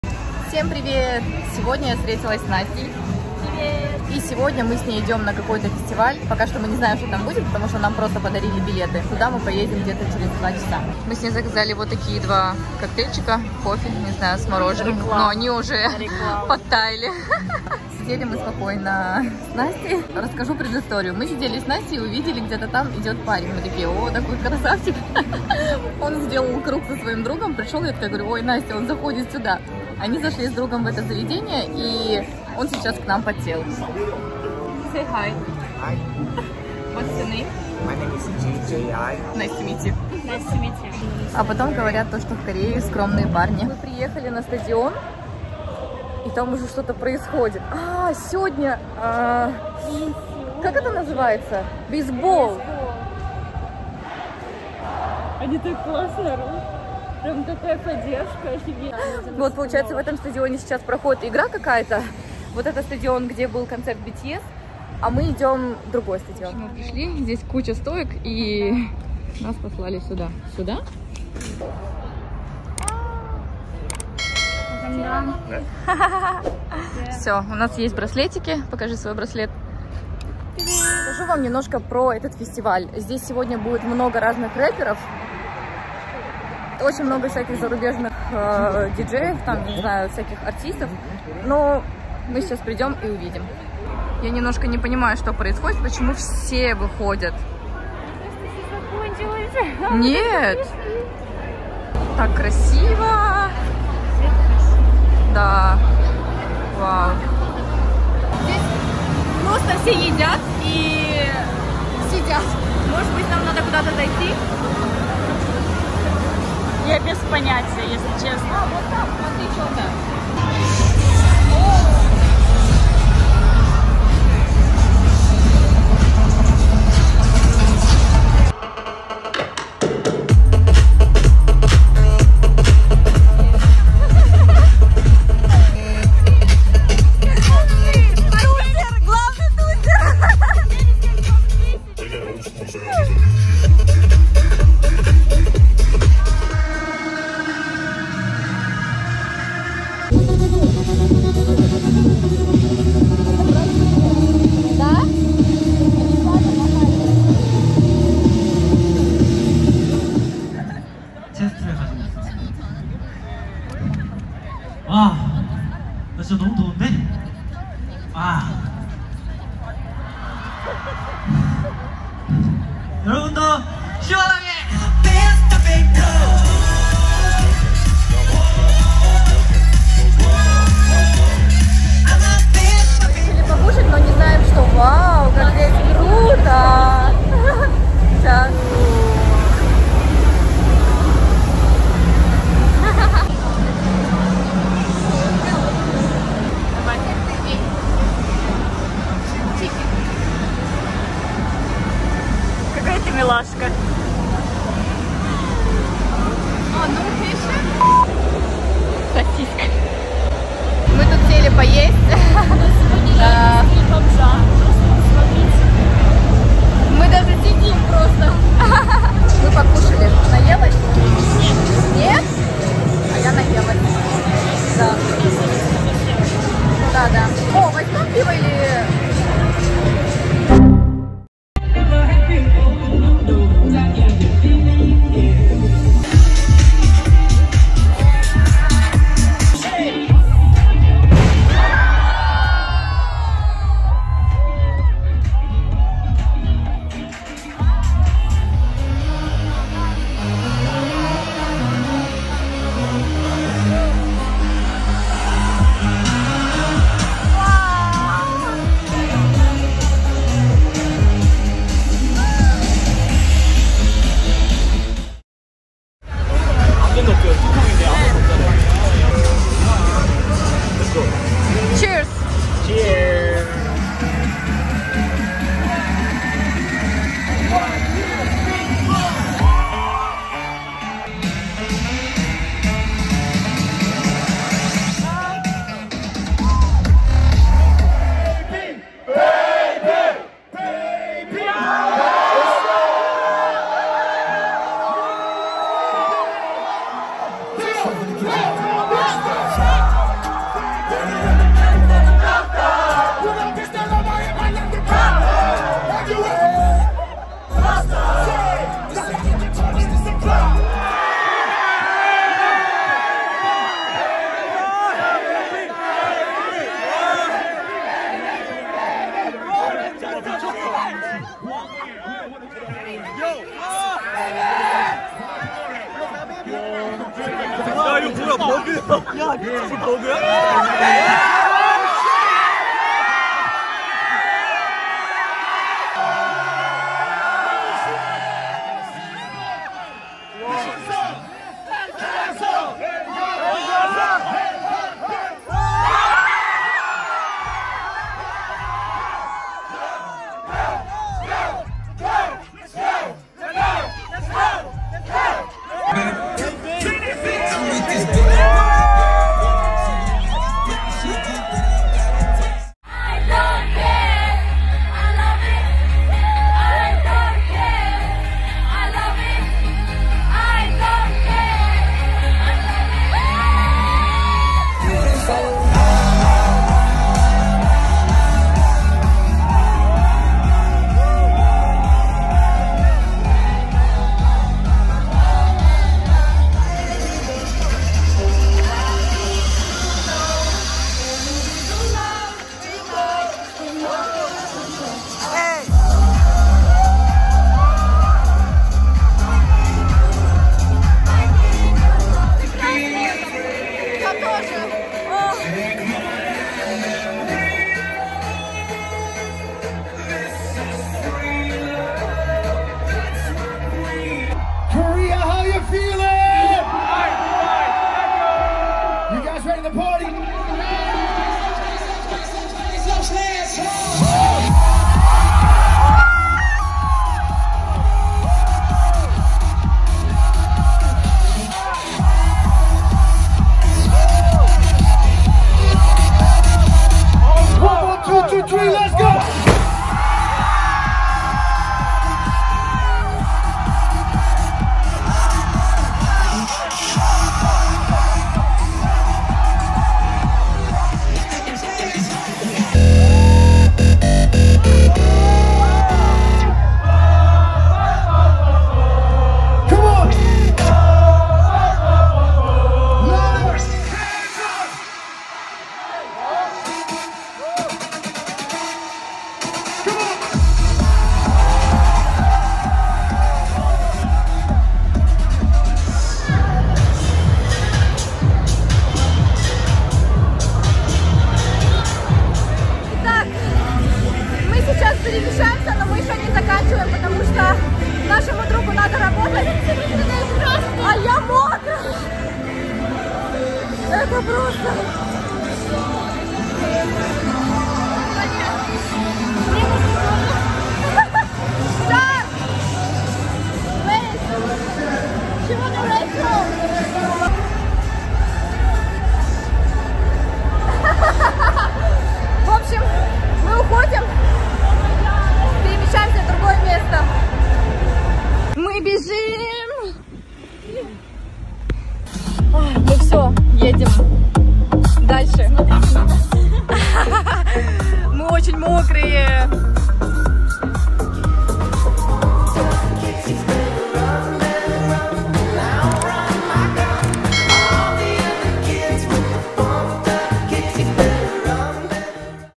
SUPERNOVA FEST SEOUL | HyunA & DAWN | DaBaby [Vlog] Первый фестиваль в моей жизни!
Фестиваль в Сеуле, с огромным количеством корейских и зарубежных исполнителей! Я впервые посетила такой фестиваль! Это было нереально круто!!